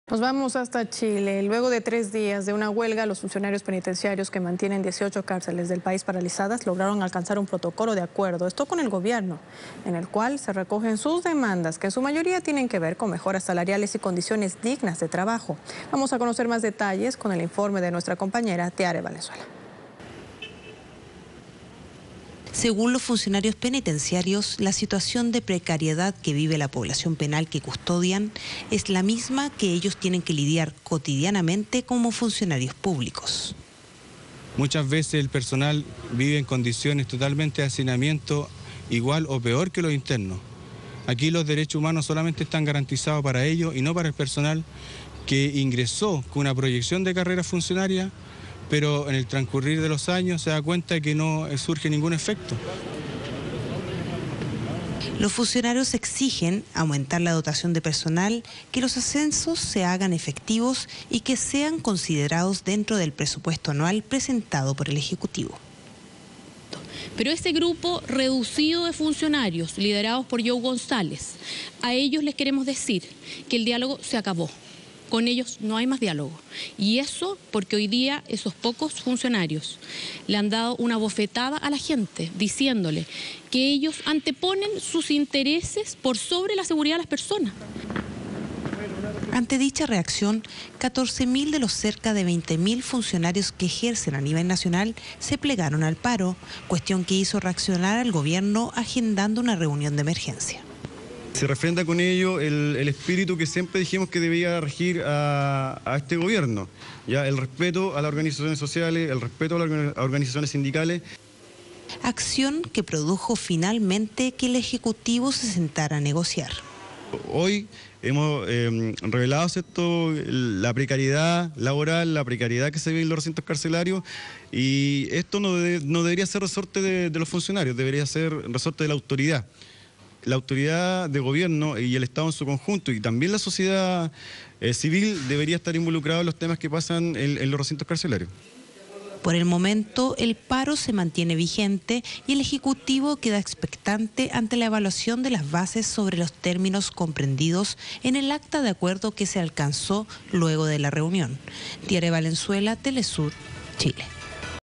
Chile: funcionários penitenciários em greve
Depois de três dias de greve, funcionários penitenciários de 18 prisões do Chile conseguiram um protocolo de acordo com o governo para atender suas demandas, centradas em melhorias salariais e condições dignas de trabalho. teleSUR